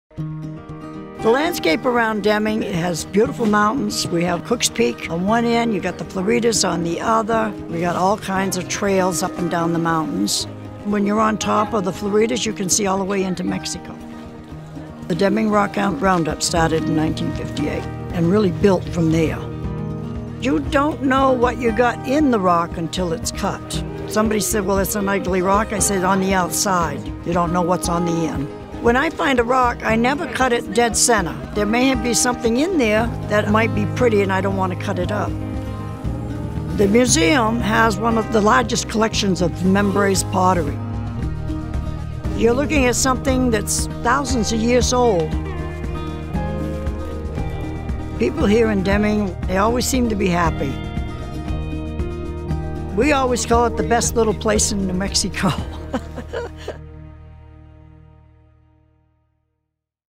Deming: "Best Little Place in New Mexico"
From Rockhounding to mountain hiking to Mimbres culture, Deming offers a visitor adventures at every turn.